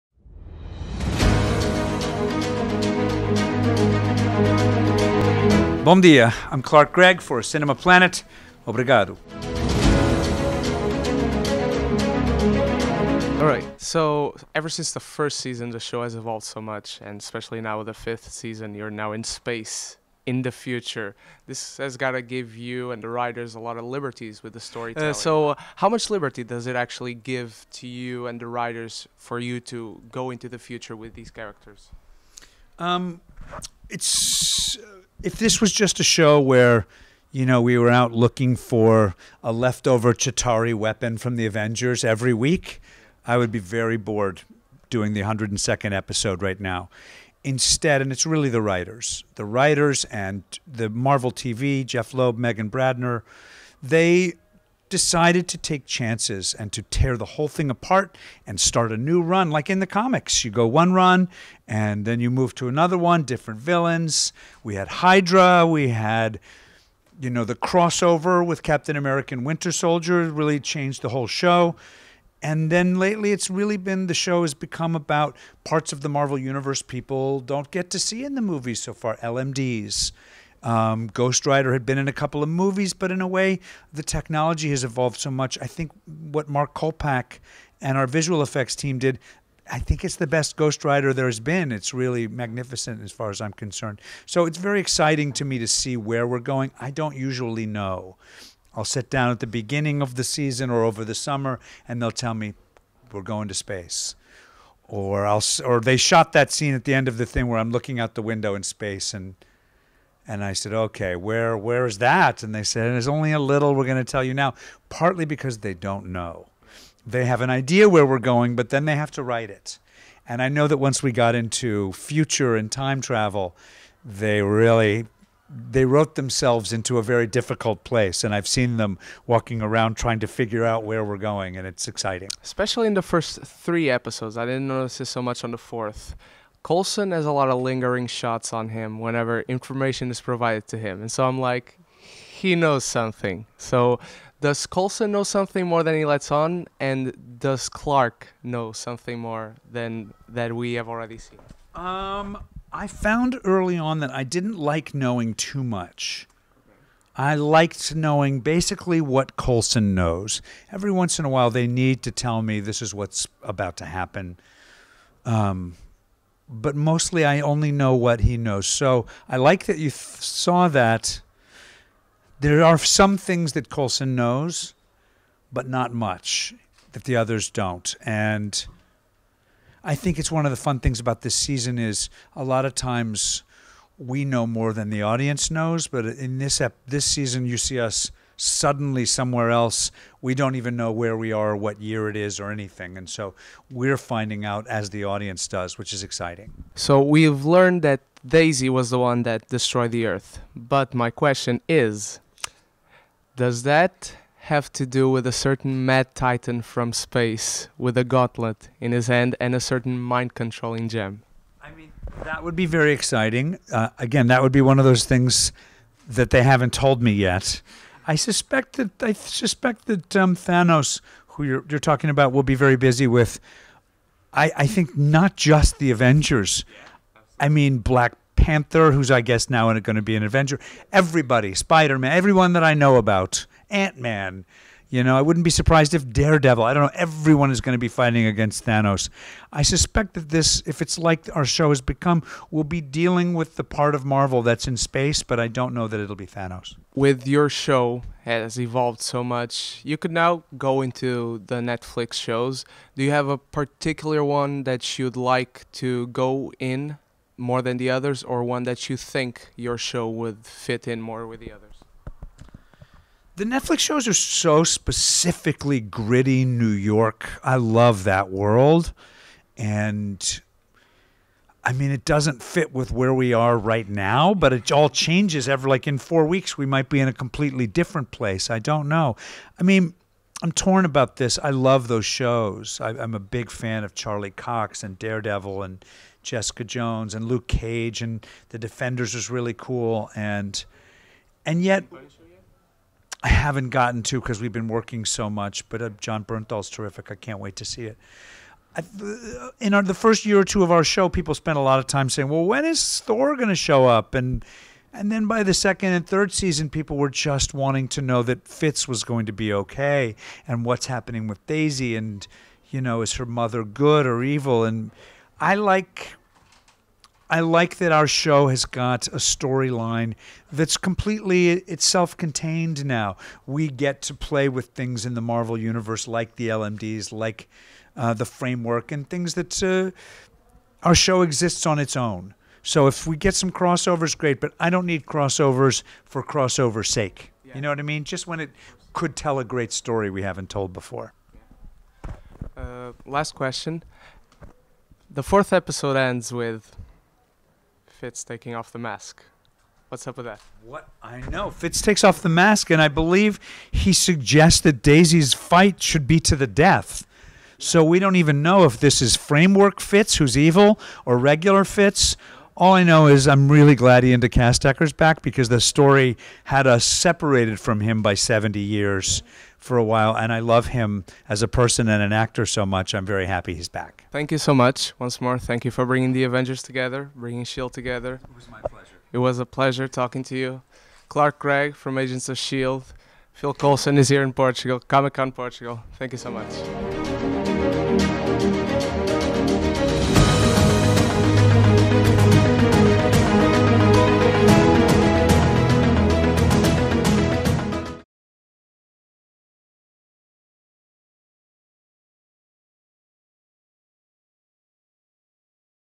Let's Talk #32 - Clark Gregg (Comic Con Portugal 2017)
Thank you Comic Con Portugal for the oportunity.